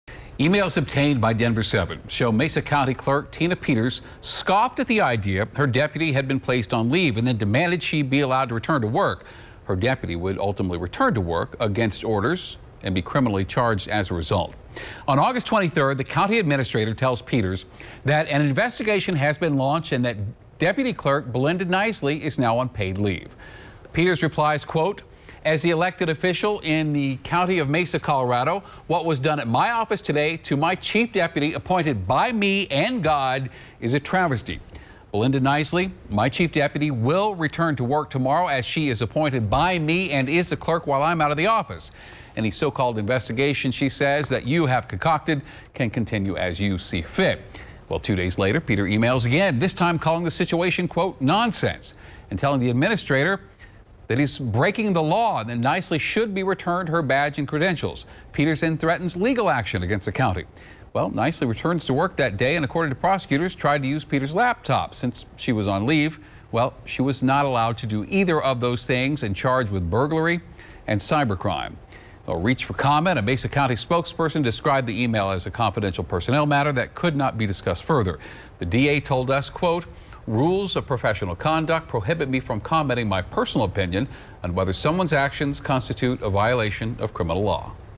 Emails show Mesa County clerk demanded suspended deputy be allowed to return to work
Emails obtained by Denver7 show Tina Peters scoffed at the idea her deputy had been placed on leave and demanded she be allowed to return to work.